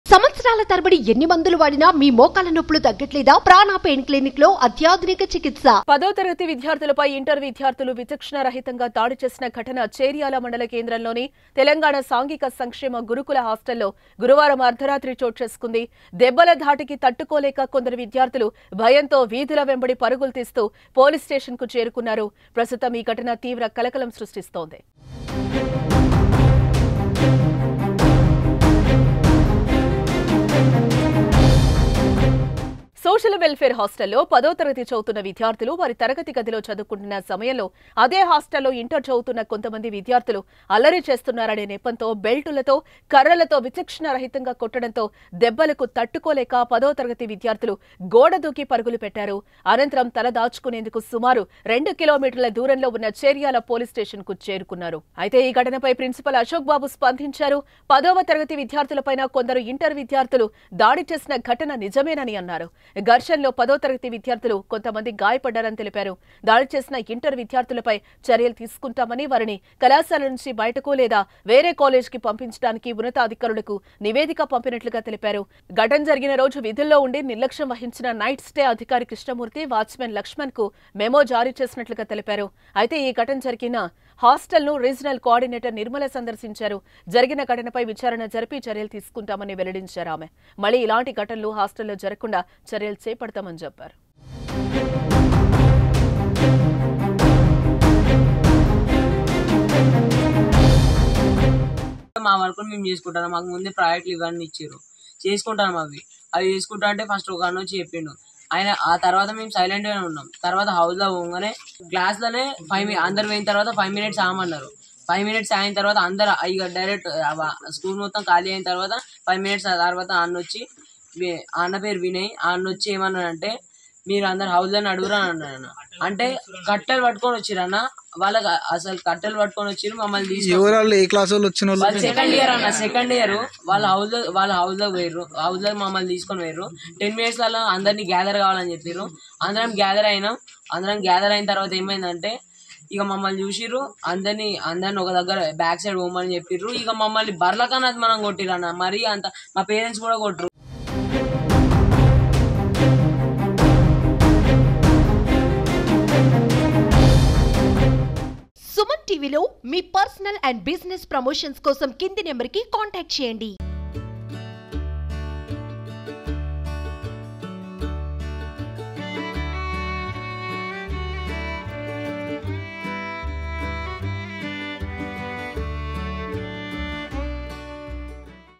పదో తరగతి విద్యార్థులపై దాడి చేసిన ఇంటర్ విద్యార్థులు | Cherial Latest Updates
Watch ►పదో తరగతి విద్యార్థులపై దాడి చేసిన ఇంటర్ విద్యార్థులు | Cherial Latest Updates | @SumanTVNalgonda